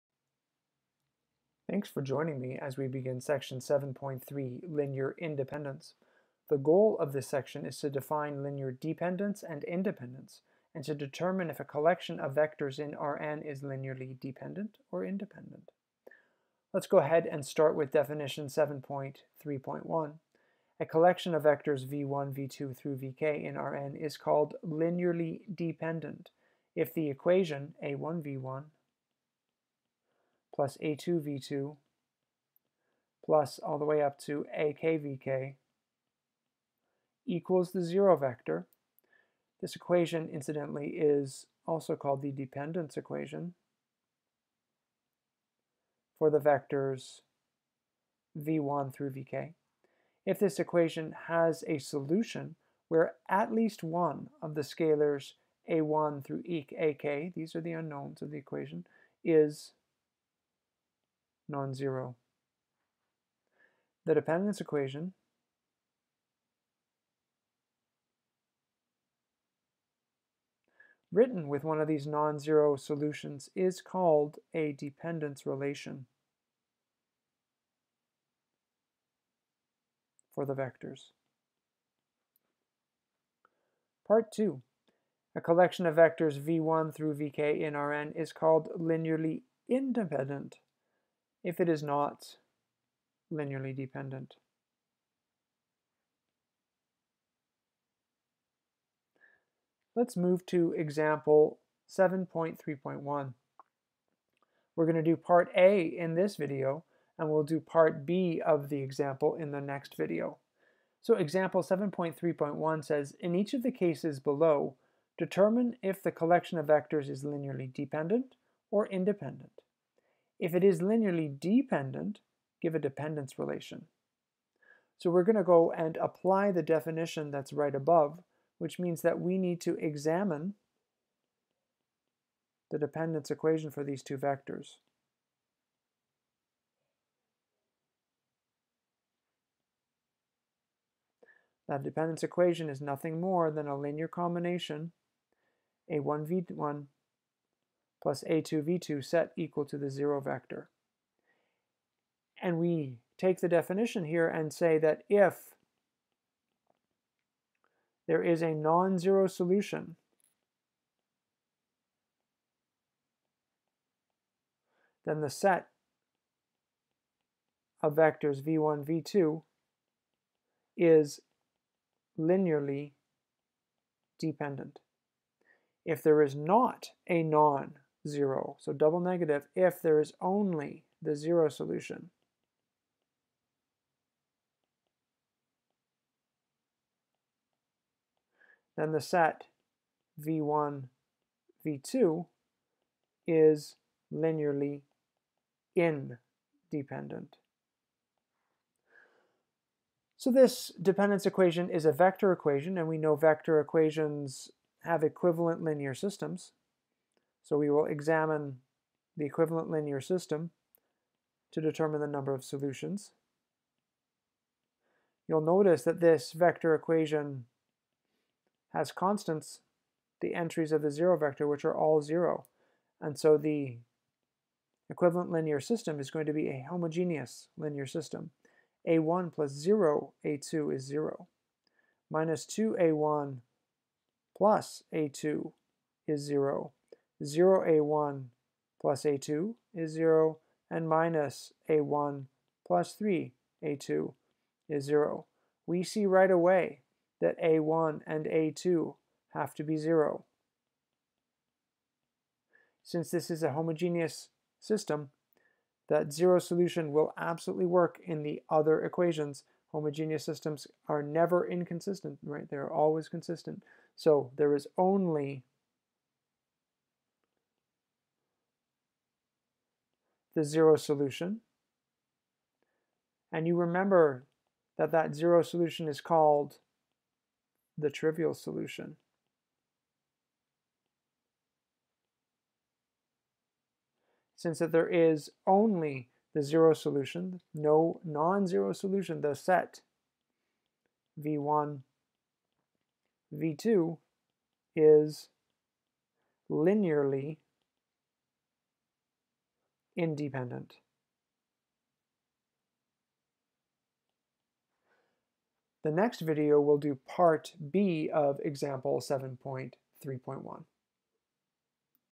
Linear Algebra 7.3 - Linear Independence (1 of 6)
It covers the following items :
7.3 Linear Independence
Definition 7.3.1
Example 7.3.1 (a)